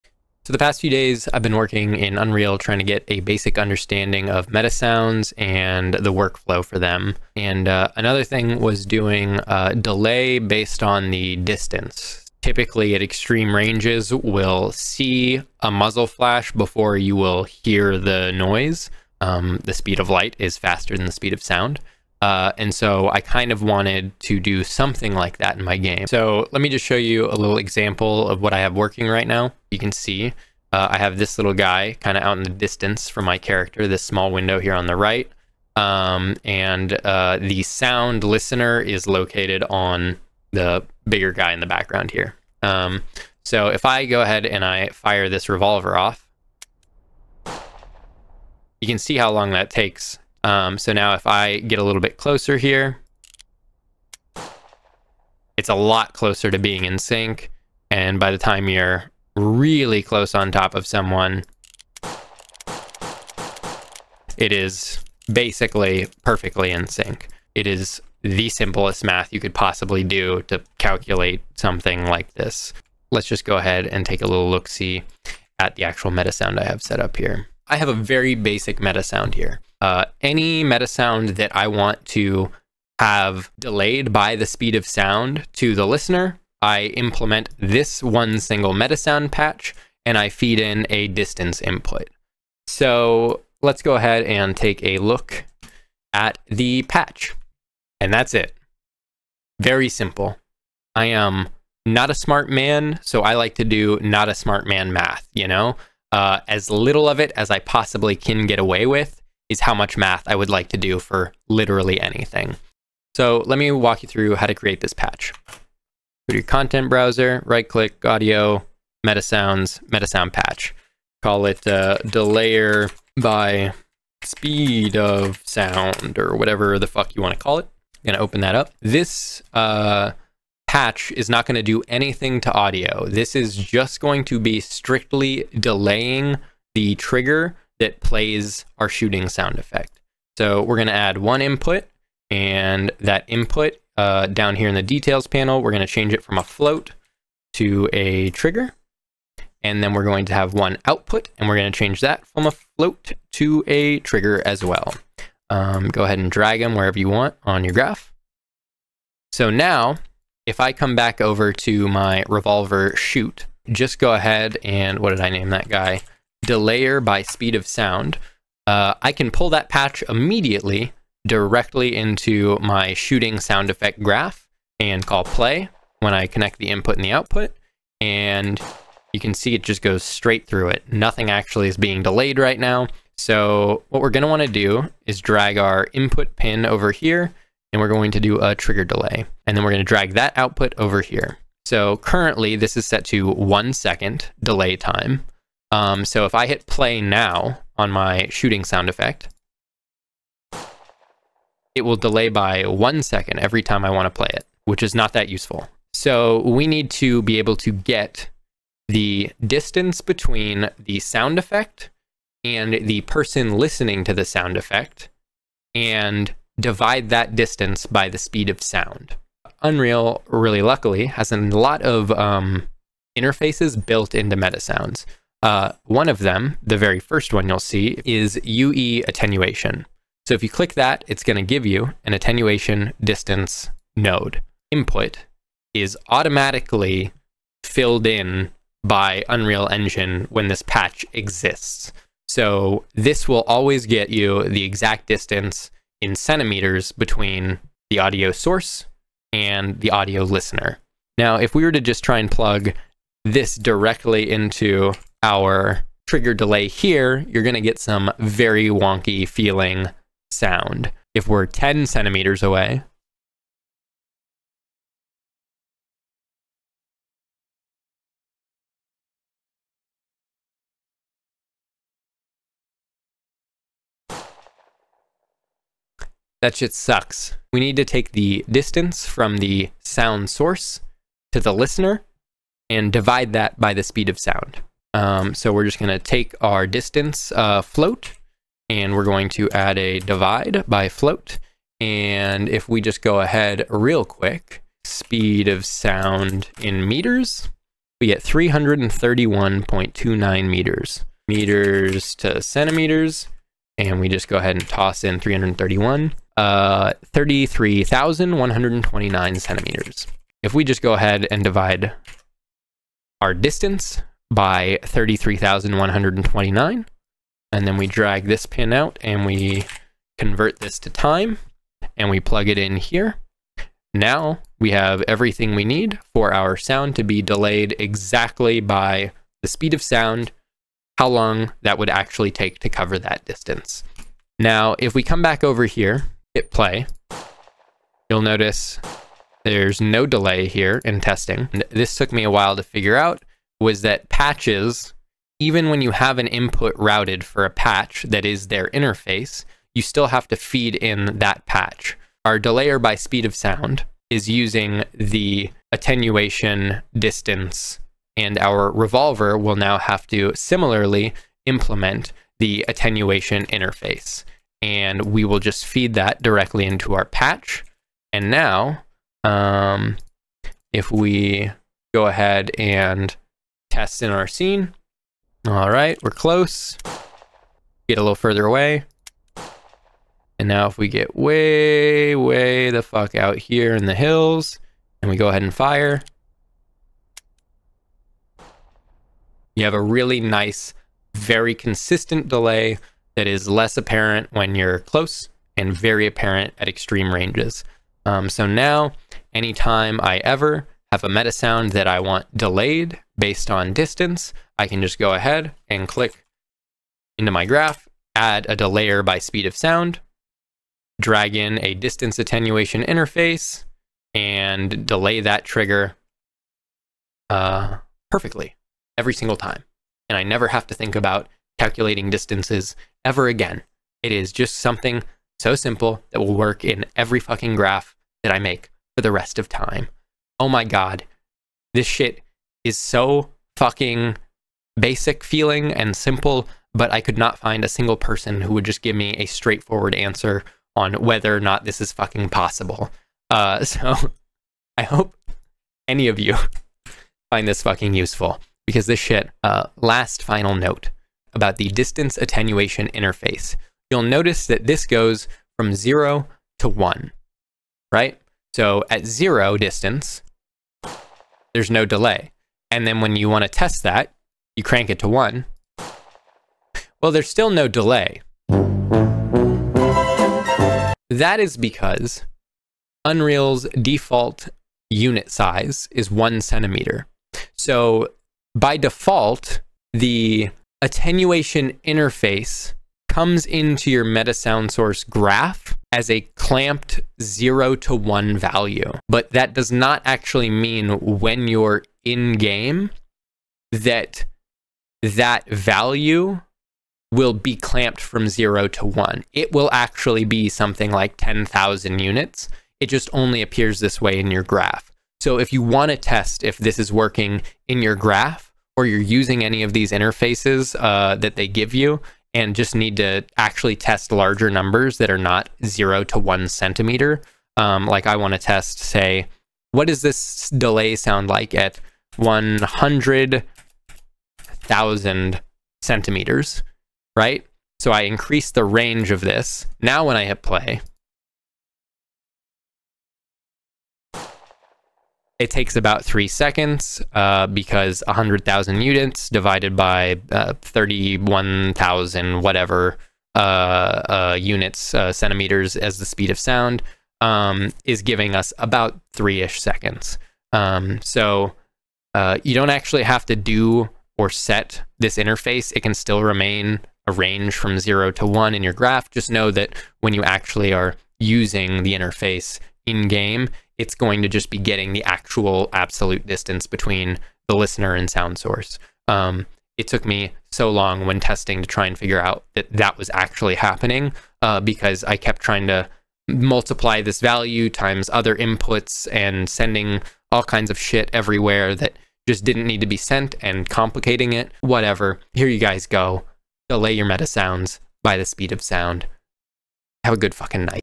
Delay your MetaSound TRIGGERS by the SPEED of sound! UE5 - Tutorial
Couldn't find a quick and easy way to implement delaying playback of a sound based on distance, so I made a patch to do it for me! Hopefully you find this useful !